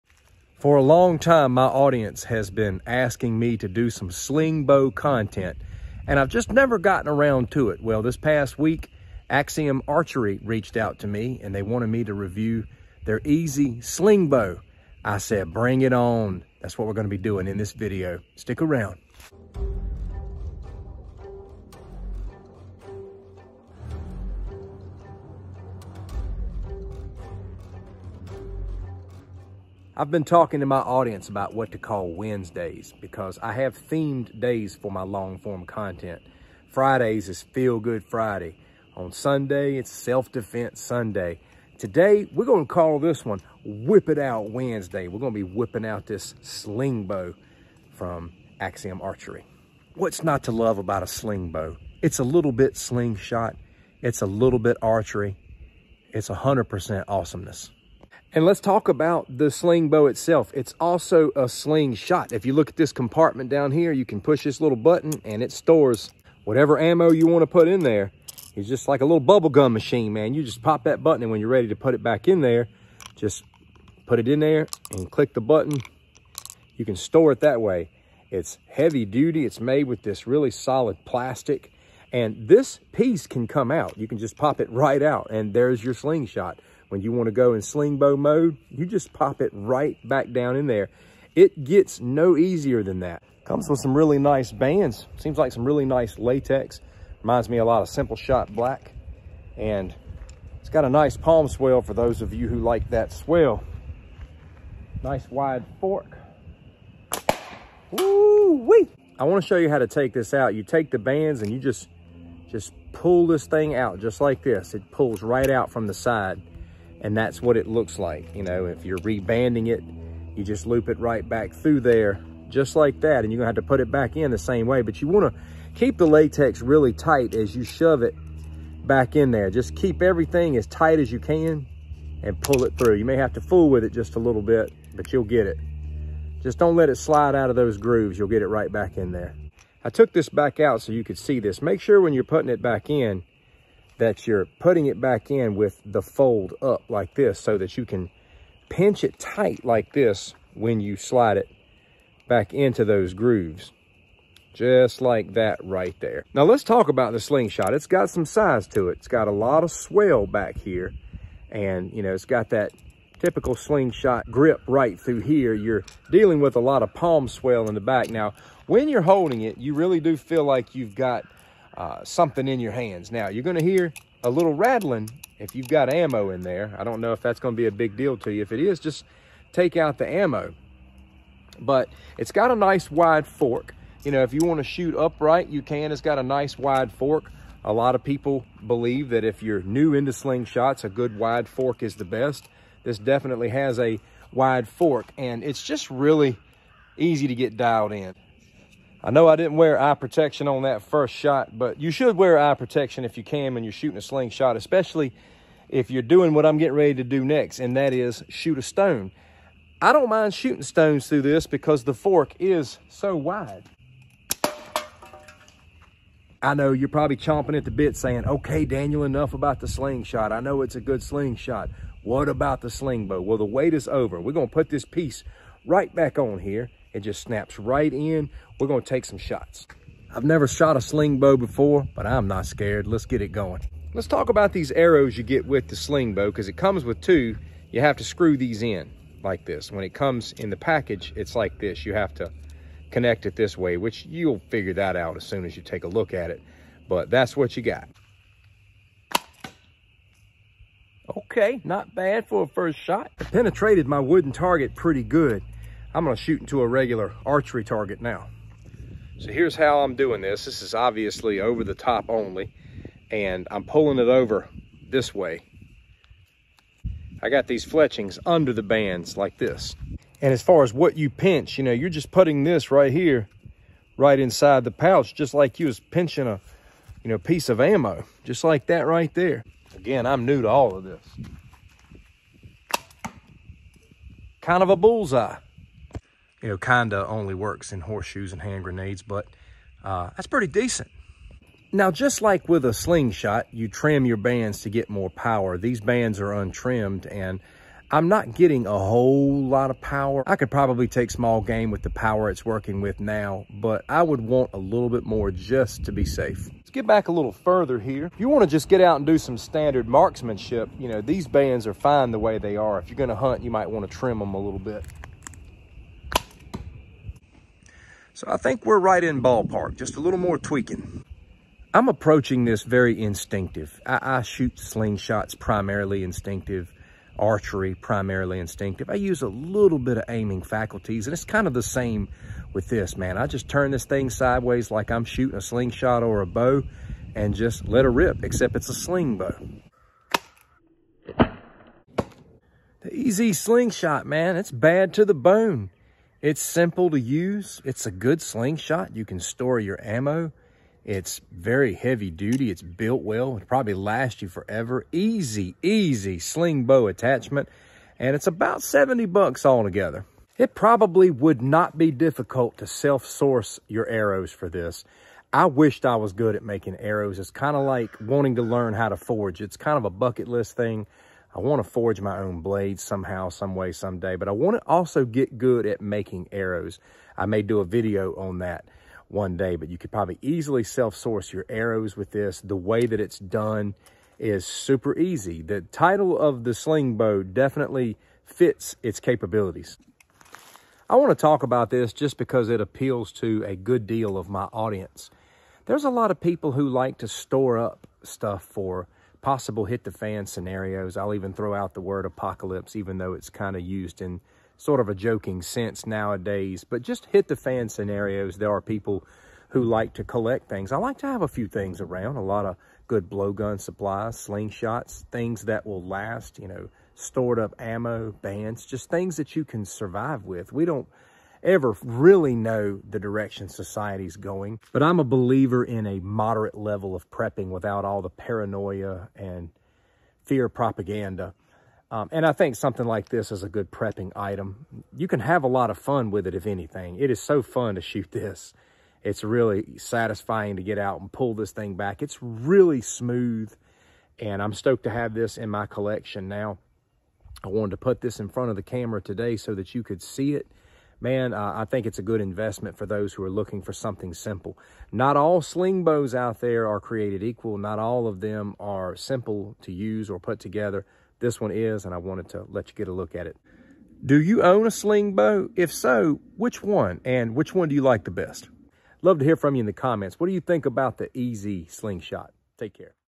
The EZ Slingbow Y from Axion Archery!
In my first ever Slingbow review, we are going to be taking a look at the EZ Slingshot Y with Slingbow from Axiom Archery. It is super simple and fun to use!

To review or purchase